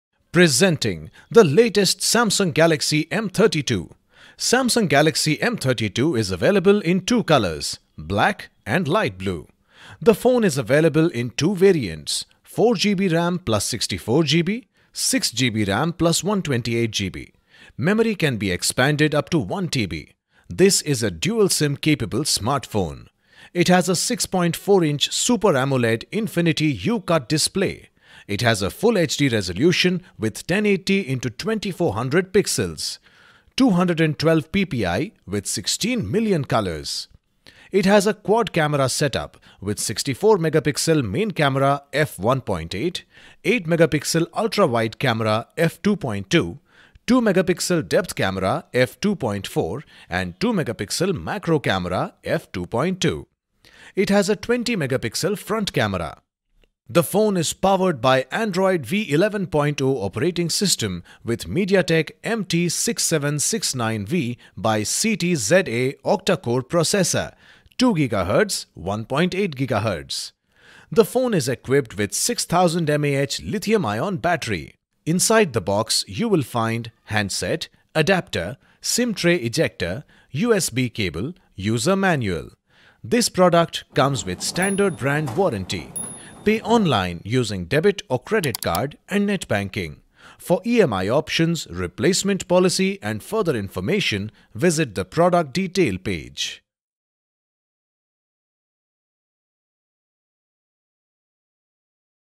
Samsung Galaxy M32 (Light Blue, 4GB RAM, 64GB Storage) 6 Months Free Screen Replacement for Prime
Segment Best 16.21 centimeters (6.4-inch) Super AMOLED - Infinity U-cut display, FHD+ resolution with 90Hz Refresh rate, 800 Nits High Brightness Mode, protected by Gorilla Glass 5
Versatile 64MP+8MP+2MP+2MP Quad camera setup-64MP (F 1.8) main camera + 8MP (F2.2) Ultra wide camera+ 2MP (F2.4) depth camera + 2MP (2.4) Macro Camera| 20MP (F2.2) front camera
Monster 6000 mAh Battery | Memory, Storage & SIM: 4GB RAM | 64GB internal memory expandable up to 1TB| SIM 1 + SIM 2 + MicroSD
Android v11.0, One UI 3.1 operating system with MediaTek Helio G80 Octa Core Processor 2GHz,1.8GHz
6000mAH lithium-ion battery, 1 year manufacturer warranty for device and 6 months manufacturer warranty for in-box accessories including batteries from the date of purchase